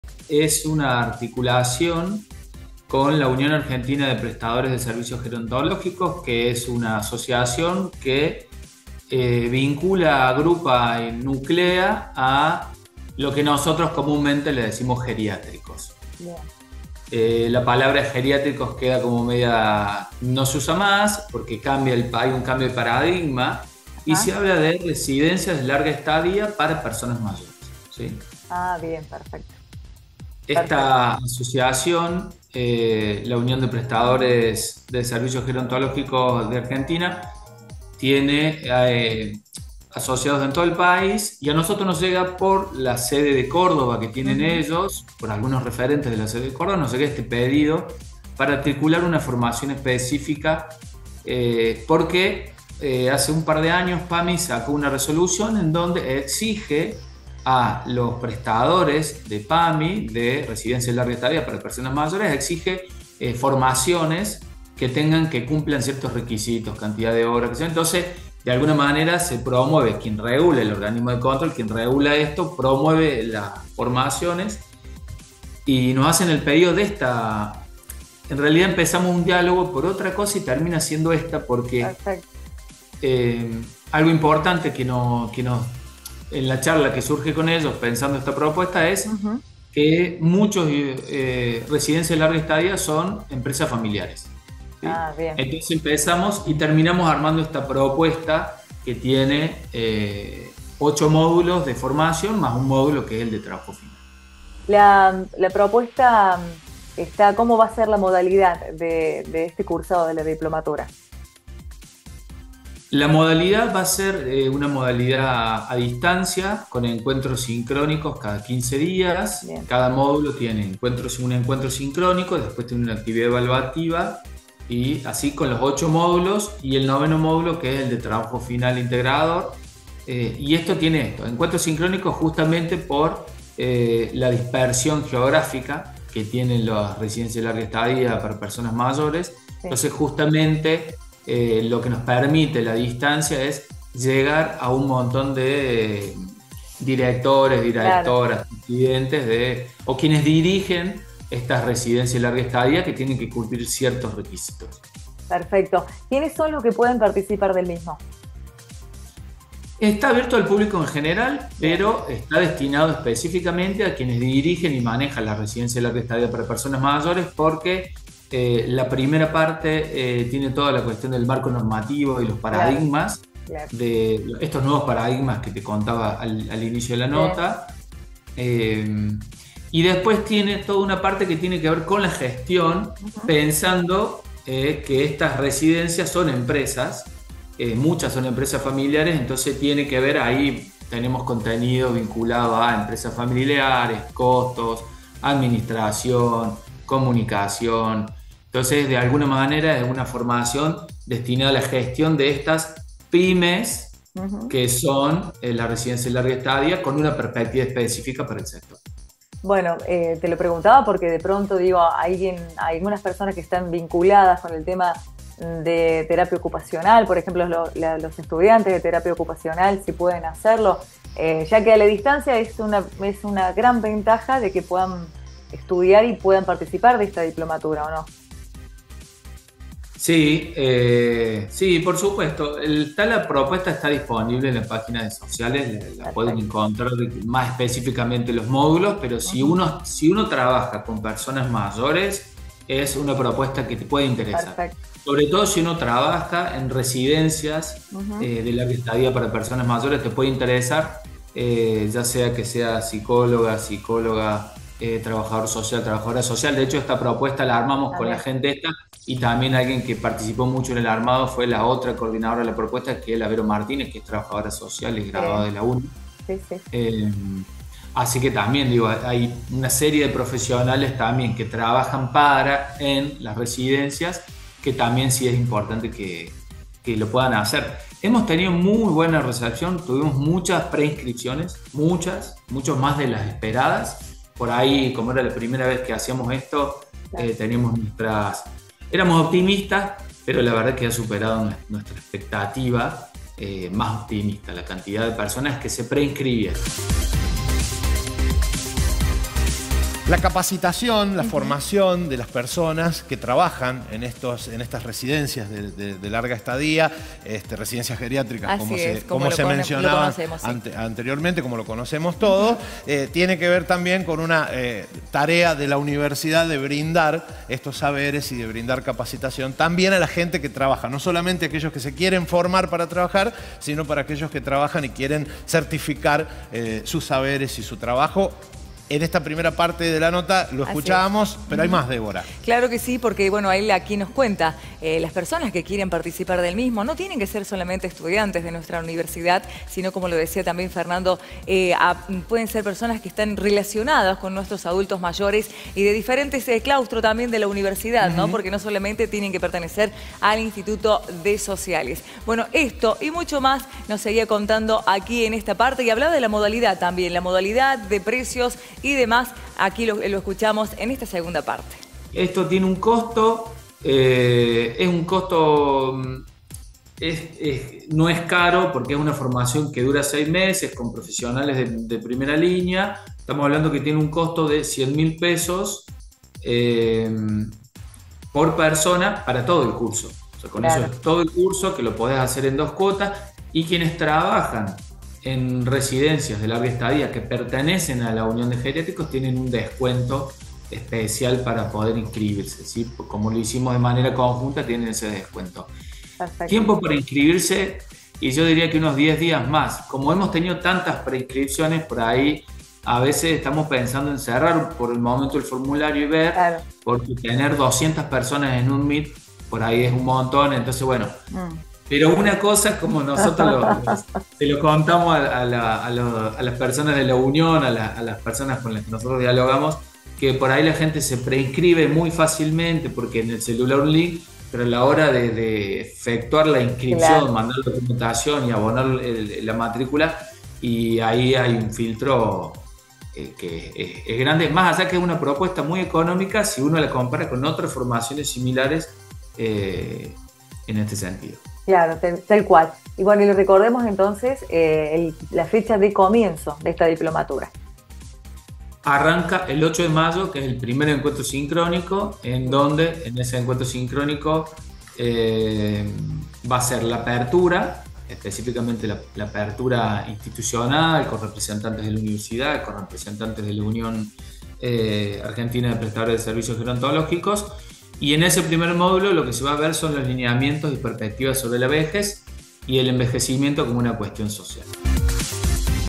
Diplomatura en Gestión de residencias de larga estadía para personas mayores
La propuesta formativa es dictada de manera conjunta por el Instituto de Ciencias Sociales de la UNVM y  la Asociación Argentina de Prestadores de Servicios Gerontológicos. Inscripciones abiertas.